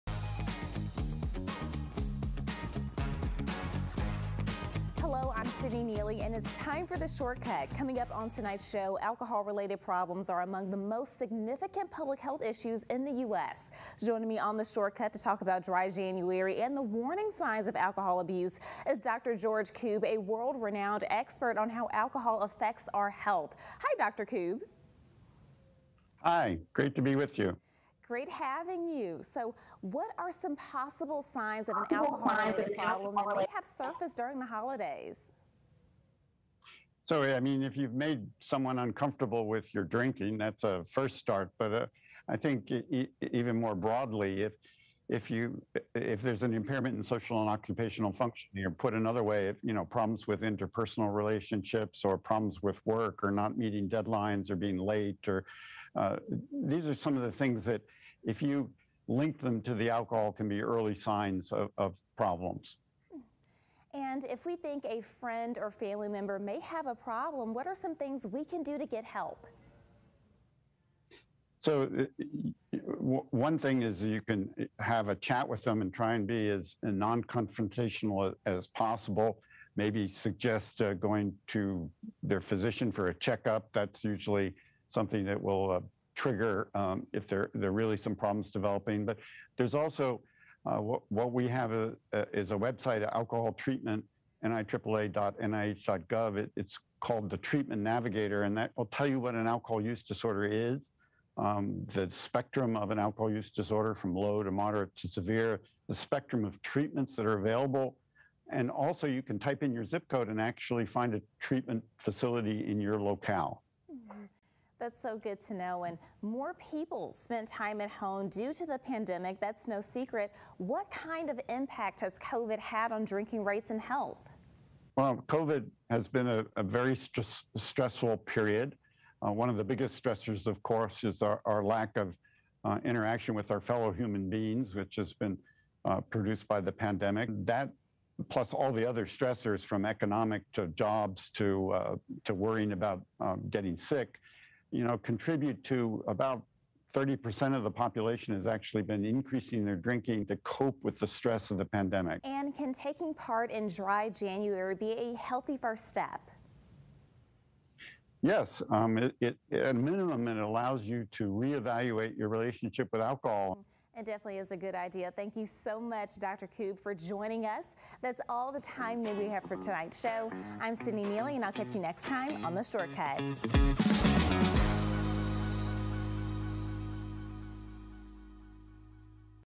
Alcohol and Dry January insight with Dr. Koob
Is taking on the Dry January challenge a good idea? Join host Sydney Neely and Dr. George Koob for an insight into the effects of alcohol and the challenge.